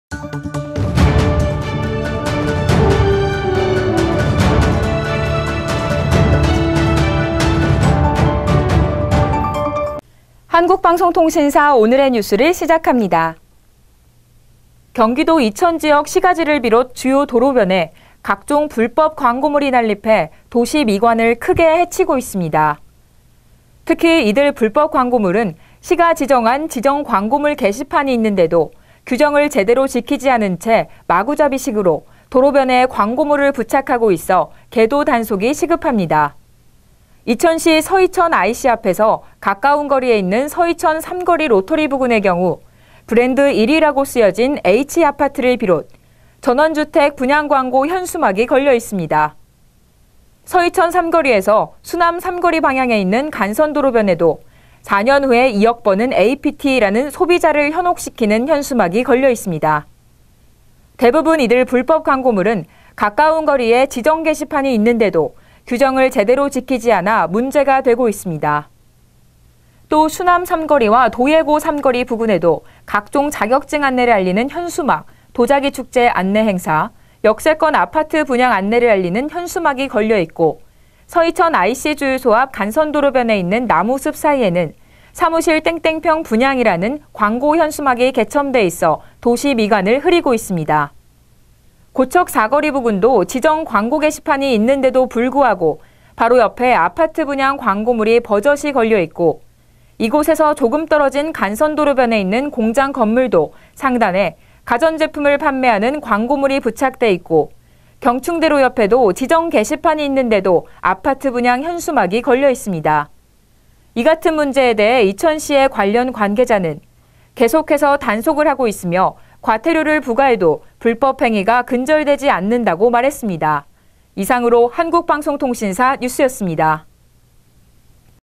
이천시 주요 도로변 불법광고물 난립
지정게시판에 광고물 부착 위한 계도 단속 시급

경기도 이천지역 시가지를 비 롯 주요 도로변에 각종 불법광고물이 난립, 도시미관을 크게 해치고 있다.

특히 이들 불법광고물은 시가 지정한 지정 광고물 게시판이 있는데도 규정을 제대로 지키지 않은 채 마구잡이식으로 도로변에 광고물을 부착하고 있어 계도 단속이 시급하다.

이천시 서 이천 I. C 앞에서 가까운 거리에 있는 서 이천 삼거리 로터리 부근의 경우 ‘브랜드 1위’라고 쓰인 H 아파트를 비 롯, 전원주택 분양광고 현수막이 걸려있다. 서 이천 삼거리에서 수남 삼거리 방향에 있는 간선도로변에도‘4년 후에 2억 버는 APT’라는 소비자를 현혹시키는 현수막이 걸려있다.

대부분 이들 불법 광고물은 가까운 거리에 지정게시판이 있는데도 규정을 제대로 지키지 않아 문제가 되고 있다
또 수남 삼거리와 도예고 삼거리 부근에도 각종 자격증 안내를 알리는 현수막, 도자기축제 안내 행사, 역세권 아파트 분양 안내를 알리는 현수막이 걸려있고, 서 이천 I. C주유소 앞 간선도로변에 있는 나무숲 사이에는 ‘사무실 00평 분양’이라는 광고 현수막이 게첨 돼 있어 도시 미관을 흐리고 있다.

고척 4거리 부근도 지정 광고 게시판이 있는데도 불구하고 바로 옆에 아파트 분양 광고물이 버젓이 걸려있고 이곳에서 조금 떨어진 간선도로변에 있는 공장 건물도 상단에 가전제품을 판매하는 광고물이 부착돼 있고, 경충대로 옆에도 지정게시판이 있는데도 아파트 분양 현수막이 걸려있다.

이 같은 문제에 대해 관련 관계자는 “계속해서 단속을 하고 있으며, 과태로를 부과해도 불법행위가 근절되지 않는다”라고 말했다.